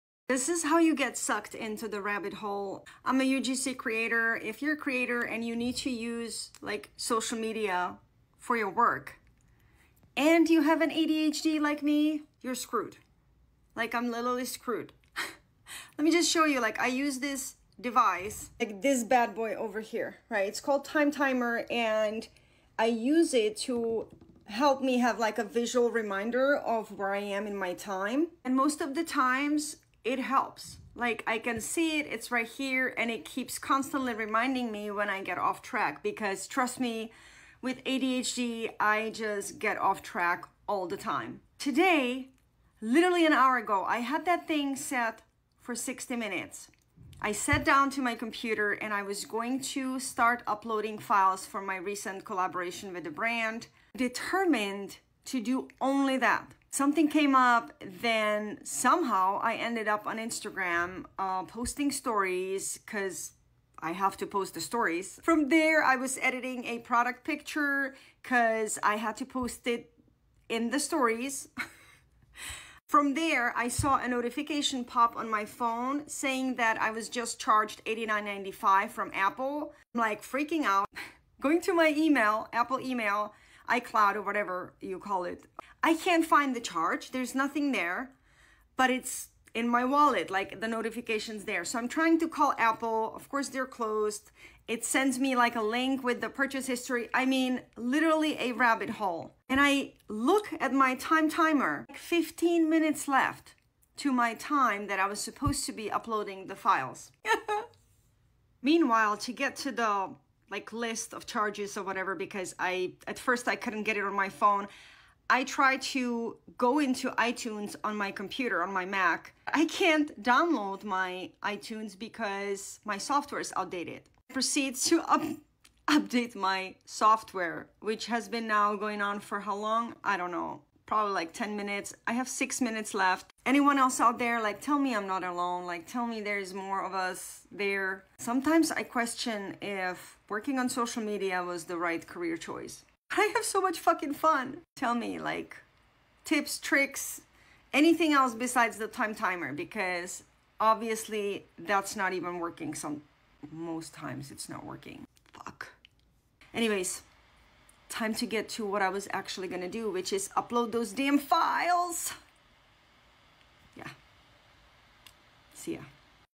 Riding the ADHD rollercoaster today! 🎢 UGC creator life is a wild ride, but wouldn't trade it for th
Riding the ADHD rollercoaster today! 🎢 UGC creator life is a wild ride, but wouldn't trade it for the world! 🎥💥 Drop your best tips for staying on task below! ⏰